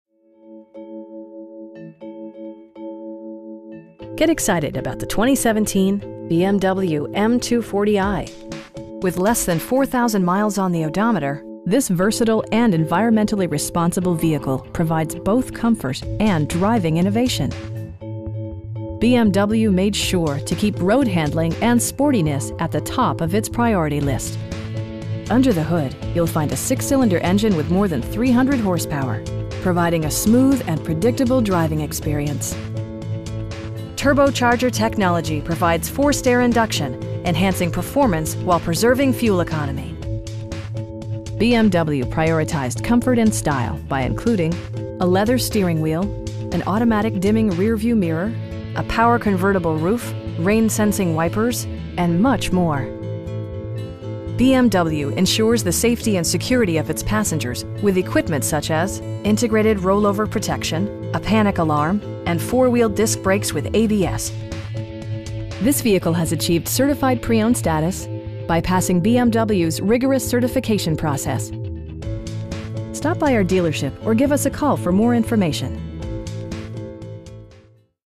2017 BMW 2 Series M240i in Winter Park, FL 32789
Get excited about the 2017 BMW M240i With less than 4,000 miles on the odometer, this versatile and environmentally responsible vehicle provides both comfort and driving innovation! BMW made sure to keep road-handling and sportiness at the top of it's priority list. Under the hood you'll find a 6 cylinder engine with more than 300 horsepower, providing a smooth and predictable driving experience. Well tuned suspension and stability control deliver a spirited, yet composed, ride and drive Turbocharger technology provides forced air induction, enhancing performance while preserving fuel economy. BMW prioritized comfort and style by including: a tachometer, a leather steering wheel, an automatic dimming rear-view mirror, front bucket seats, a power convertible roof, rain sensing wipers, and much more. Everything is where it ought to be, from the dashboard controls to the door locks and window controls. With high intensity discharge headlights illuminating your path, you'll always appreciate maximum visibility. BMW ensures the safety and security of its passengers with equipment such as: dual front impact airbags, integrated roll-over protection, traction control, brake assist, anti-whiplash front head restraints, a panic alarm, an emergency communication system, and 4 wheel disc brakes with ABS. This vehicle has achieved Certified Pre-Owned status, by passing BMW's rigorous certification process. We know that you have high expectations, and we enjoy the challenge of meeting and exceeding them! Stop by our dealership or give us a call for more information.